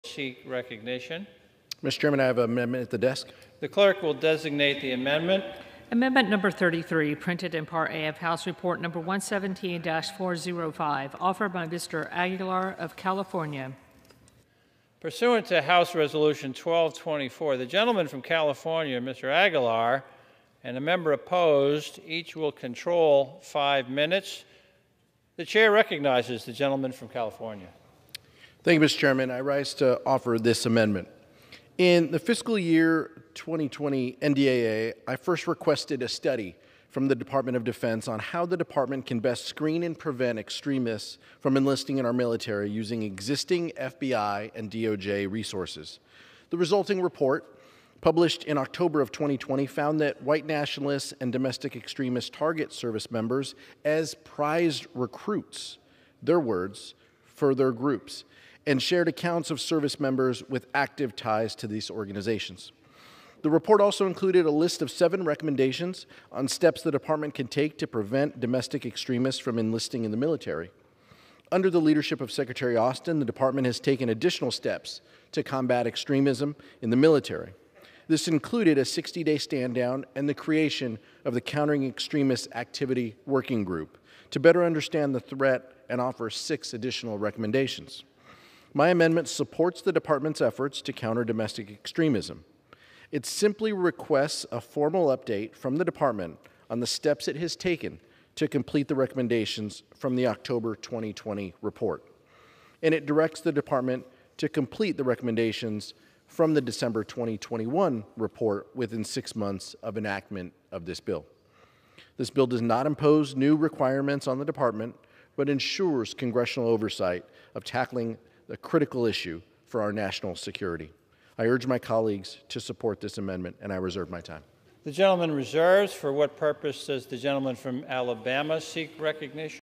Pete Aguilar Stresses Importance Of Countering Domestic Extremism Inside Military
Rep. Pete Aguilar (D-CA) speaks on the House floor about his amendment to combat extremist recruiting in the military.